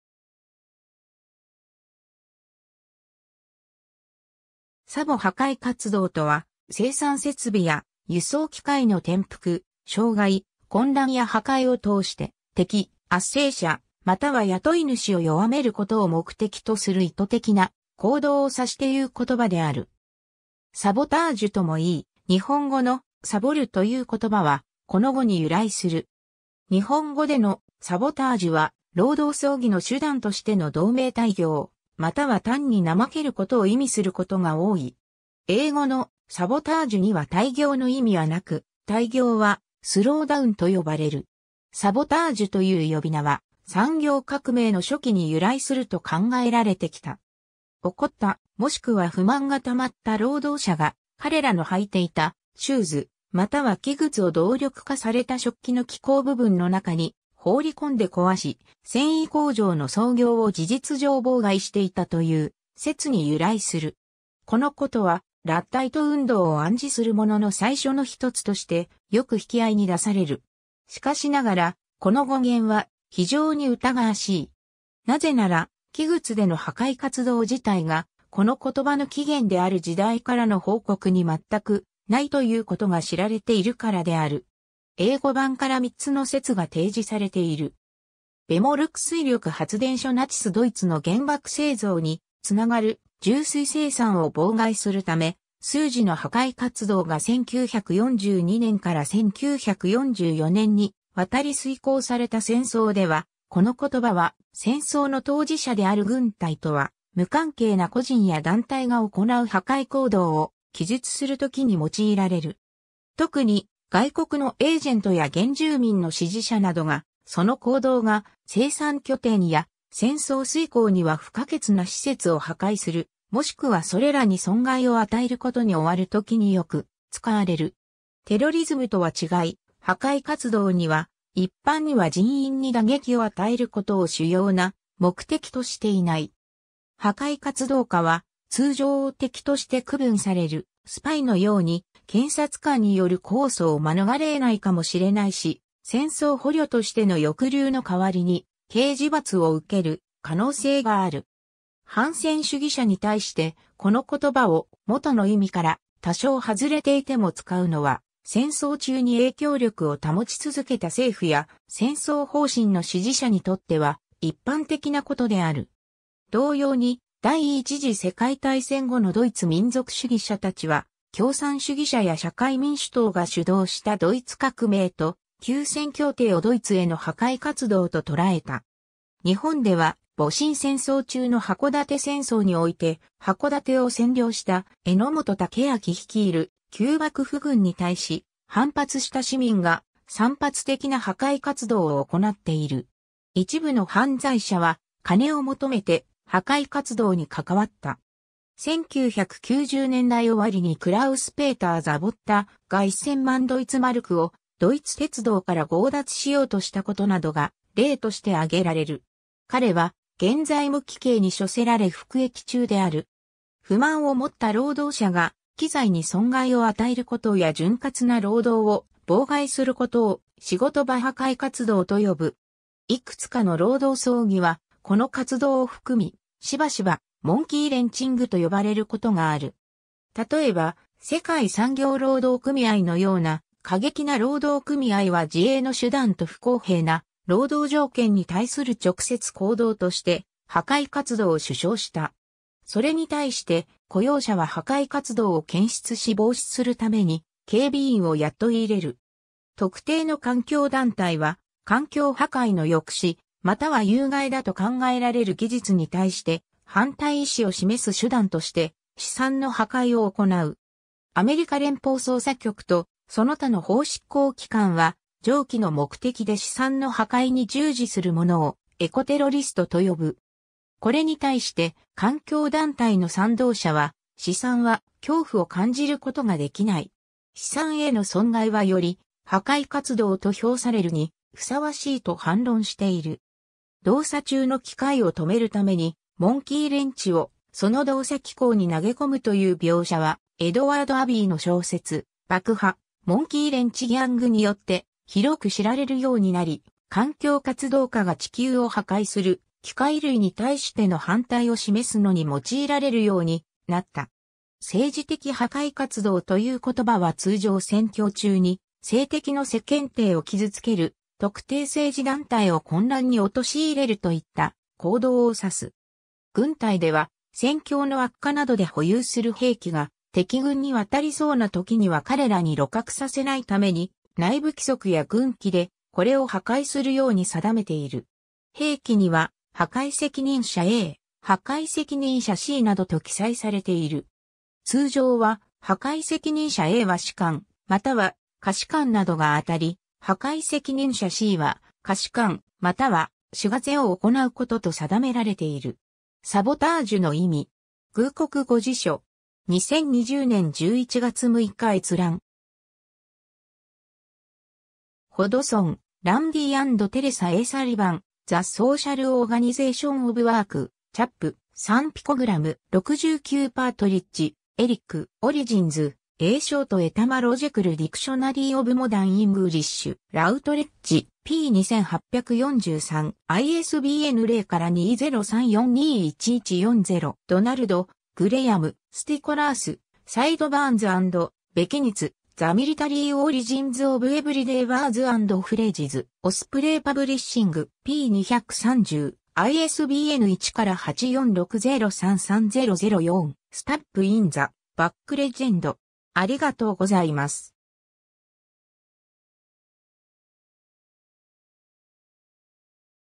破壊活動
サボ（木靴） 破壊活動（はかいかつどう、フランス語: sabotage）とは、生産設備や輸送機械の転覆、障害、混乱や破壊を通して敵、圧制者または雇い主を弱めることを目的とする意図的な行動をさしていう言葉である。
サボタージュともいい、日本語の「サボる」という言葉は、この語に由来する。
 日本語での「サボタージュ」は、労働争議の手段としての同盟怠業（どうめいたいぎょう）、または単に怠けることを意味することが多い。
英語の「sabotage」には怠業の意味はなく、怠業は「slowdown」と呼ばれる。
 サボタージュという呼び名は、産業革命の初期に由来すると考えられてきた。
怒った、若しくは不満がたまった労働者が、彼らの履いていたシューズまたは木靴（フランスでは「サボ」と呼ばれていた）を動力化された織機の機構部分の中に放りこんで壊し、繊維工場の操業を事実上妨害していたという説に由来する。
このことは、ラッダイト運動を暗示するものの最初の一つとして、良く引き合いに出される。
しかしながら、この語源は非常に疑わしい。
何故なら、木靴での破壊活動自体が、この言葉の起源である時代からの報告に全くないということが知られているからである。
 英語版から3つの説が提示されている。
 ヴェモルク水力発電所（ノルウェー南部のテレマルク県） ナチス・ドイツの原爆製造につながる重水生産を妨害するため、数次の破壊活動 (ノルスク・ハイドロ重水工場破壊工作) が1942年から1944年にわたり遂行された 戦争では、この言葉は、戦争の当事者である軍隊とは無関係な個人や団体が行う破壊行動を記述するときに用いられる。
特に、外国のエージェントや原住民の支持者などが、その行動が生産拠点や戦争遂行には不可欠な施設（例えば器材、工場、ダム、公共事業、貯蔵プラント、物流経路）を破壊する、もしくはそれらに損害をあたえることに終わるときに良く使われる。
テロリズムとは違い、破壊活動には、一般には人員に打撃を与えることを主要な目的としていない。
破壊活動家は通常敵として区分される、スパイのように、検察官による公訴を免れえないかもしれないし、戦争捕虜としての抑留の代わりに刑事罰を受ける可能性がある。
 反戦主義者に対して、この言葉を元の意味から多少外れていても使うのは、戦争中に影響力を保ち続けた政府や戦争方針の支持者にとっては一般的なことである。
同様に、第一次世界大戦後のドイツ民族主義者たちは、共産主義者や社会民主党 (SPD)が主導したドイツ革命と休戦協定（いわゆる背後の一突き）をドイツへの破壊活動と捉えた。
 日本では、戊辰戦争中の箱館戦争において、箱館（現在の北海道函館市）を占領した榎本武揚率いる旧幕府軍に対し、反発した市民が散発的な破壊活動を行なっている。
 一部の犯罪者は、金を求めて破壊活動に関わった。
1990年代終わりにクラウス＝ペーター・ザボッタが一千万ドイツマルクをドイツ鉄道から強奪しようとしたことなどが例として挙げられる。
彼は現在無期刑に処せられ服役中である。
 不満を持った労働者が、機材に損害を与えることや潤滑な労働を妨害することを「仕事場破壊活動」と呼ぶ。
いくつかの労働争議はこの活動を含み、しばしば「モンキーレンチング」と呼ばれることがある。
例えば、世界産業労働組合のような、過激な労働組合は自衛の手段と不公平な労働条件に対する直接行動として破壊活動を主唱した。
それに対して、雇用者は破壊活動を検出し防止するために警備員を雇い入れる。
 特定の環境団体は、環境破壊の抑止または有害だと考えられる技術に対して反対意思を示す手段として資産の破壊を行う。
アメリカ連邦捜査局とその他の法執行機関は上記の目的で資産の破壊に従事する者を「エコテロリスト」と呼ぶ。
これに対して、環境団体の賛同者は、「資産は恐怖を感じることが出来ない。資産への損害はより、“破壊活動”と評されるにふさわしい」と反論している。
動作中の機械を止めるために、モンキーレンチをその動作機構に投げ込むという描写は、エドワード・アビーの小説「爆破―モンキーレンチギャング」（築地書院　2001年）によって広く知られるようになり、環境活動家が地球を破壊する機械類に対しての反対を示すのに用いられるようになった。
 「政治的破壊活動」という言葉は通常選挙中に政敵の世間体を傷つける、特定政治団体を混乱に陥れるといった行動を指す。
 軍隊では、戦況の悪化などで保有する兵器が敵軍に渡りそうな時には彼らに鹵獲させな...